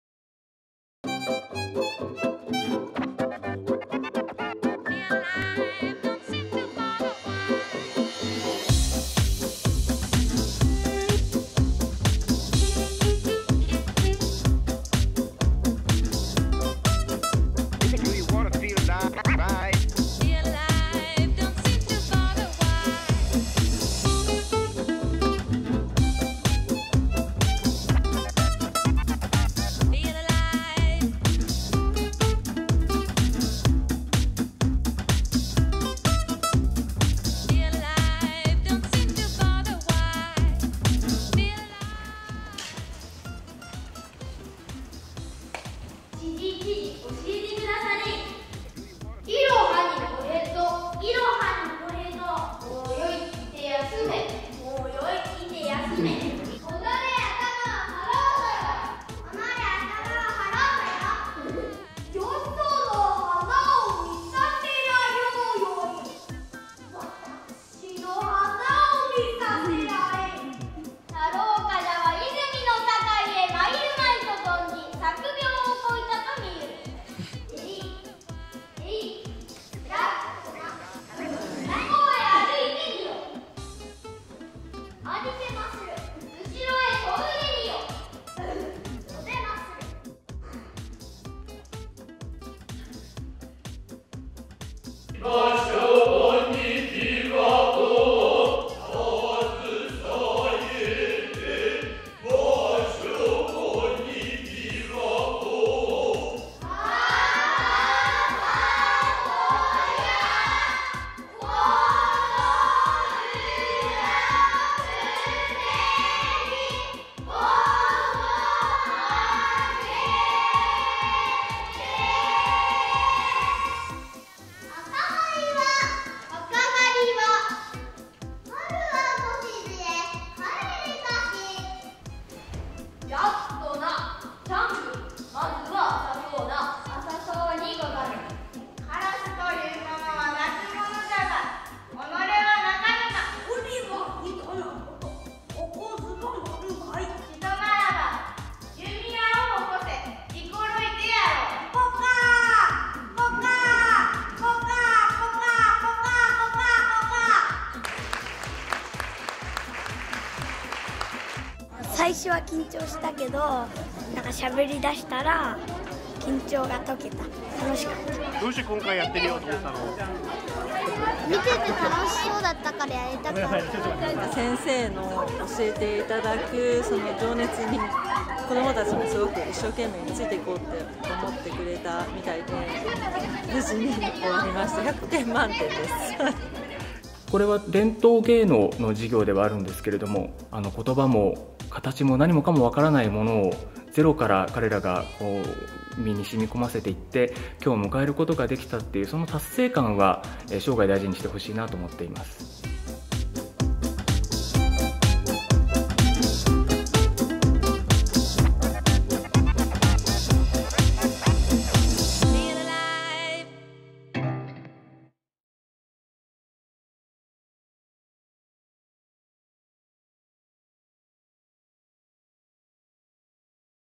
2017年度 キッズ伝統芸能体験（ショートバージョン）/ FY2017 Traditional Performing Arts for Kids (Short version)
「キッズ伝統芸能体験」は、能楽、日本舞踊、三曲、長唄の一流の実演家から、子供たちが数か月にわたり直接指導を受け、最後にその成果を舞台で発表するプログラムです。今回は能楽を学んできた子供たちの発表会の様子をご紹介します。

主催：東京都、アーツカウンシル東京（公益財団法人東京都歴史文化財団）、公益社団法人日本芸能実演家団体協議会

Organized by Tokyo Metropolitan Government, Arts Council Tokyo (Tokyo Metropolitan Foundation for History and Culture), Japan Council of Performers Rights & Performing Arts Organizations (Geidankyo)